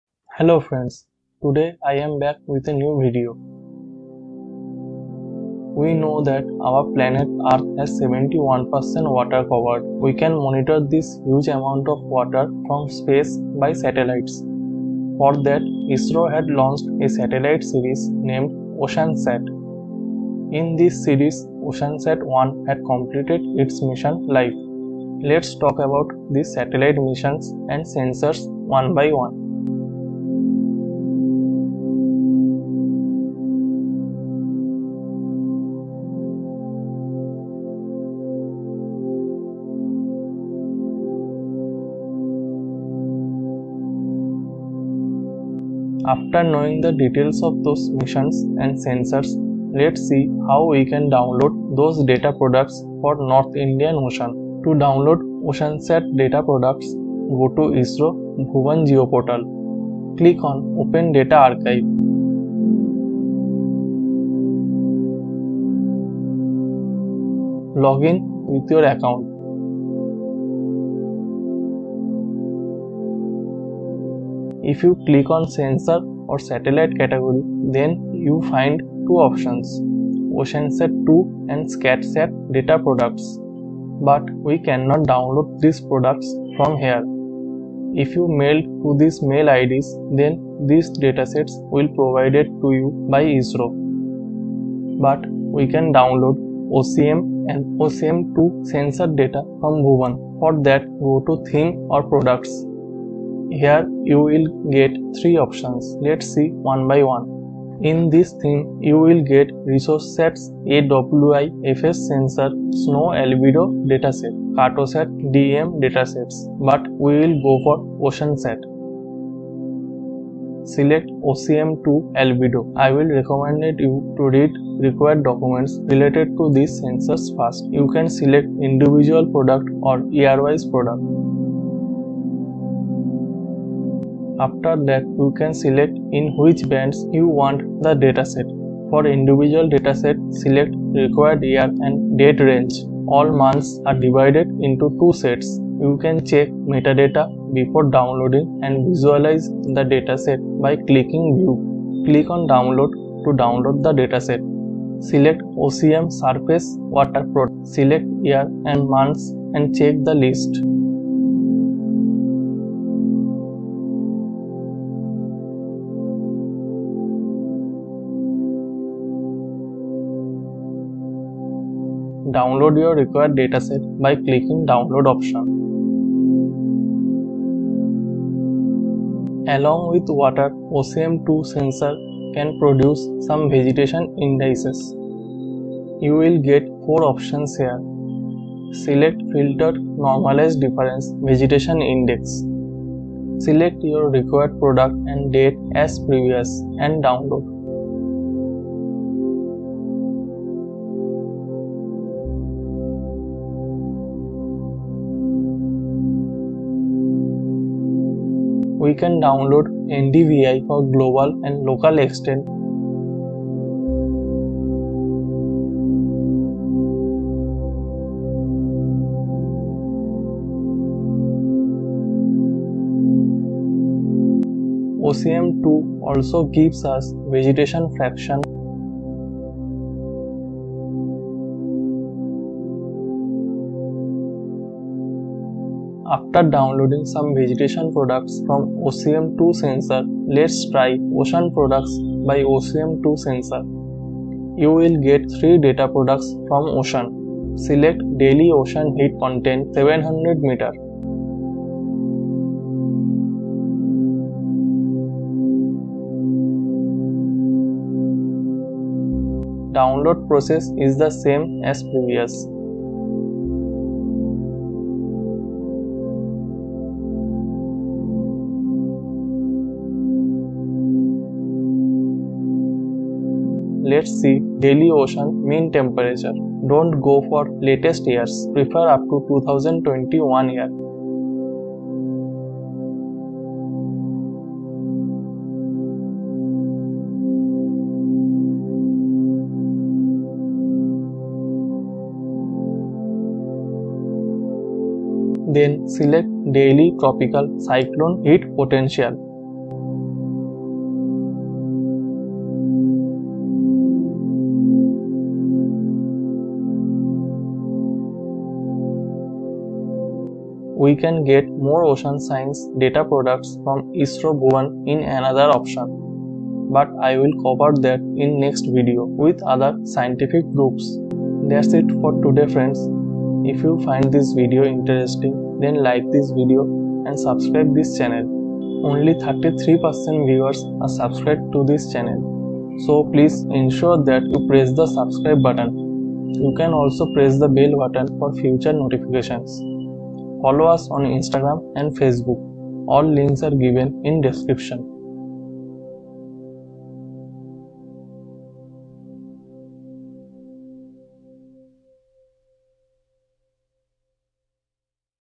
Download OceanSat Data products fom ISRO Bhuvan | OceanSat OCM Sensor
Hi,
Welcome to Dawn of Geospatial World. Where you can learn about GIS, Remote sensing, Satellite Imageries.

Today we will see how we can download OceanSat satellite's OSM sensor data products.

OceanSat is a satellite series launched by ISRO, India in 1999. This series has three main satellites. OceanSat 1 had completed its mission and OceanSat 2, OceanSat 3 are still in operation. With these satellites, ScatSat satellite is also included.
OceanSat has a Primary OCM sensor monitoring different ocean, water, vegetation parameters. ISRO had collected those data and prepare a complete database of those images. We can view and download Ocean heat content, mean temperature, surface water, albedo, NDVI, Vegetation fraction from OCM sensor.